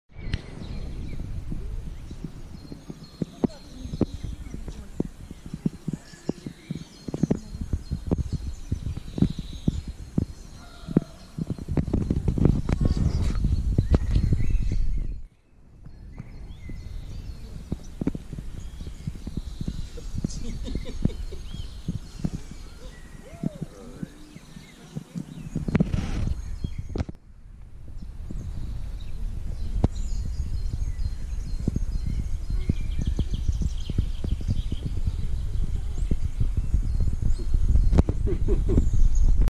Bust of Peter Wittgenstein in Camenca [CC]
The statue is located in the city of Camenca, Transnistria.